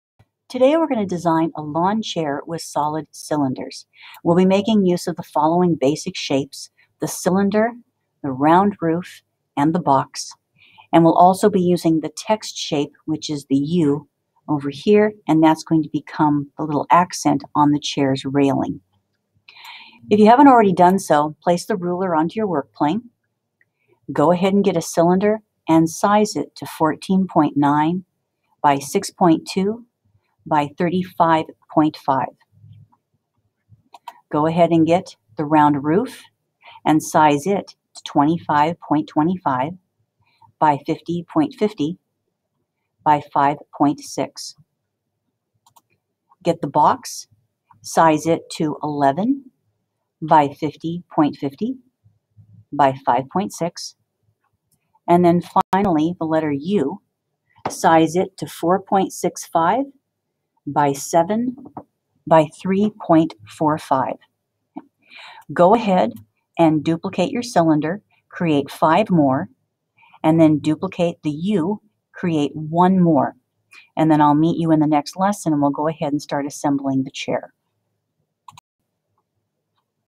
1) Lawn Chair With Solid Cylinders, TinkerCad Tutorial
In this video, we'll select and size the basic pieces needed to build the lawn chair. You'll be using the cylinder, round roof, box shape and the letter "u". In the second lesson, we'll focus on positioning and duplicating the cylinders to model the chair's cushion.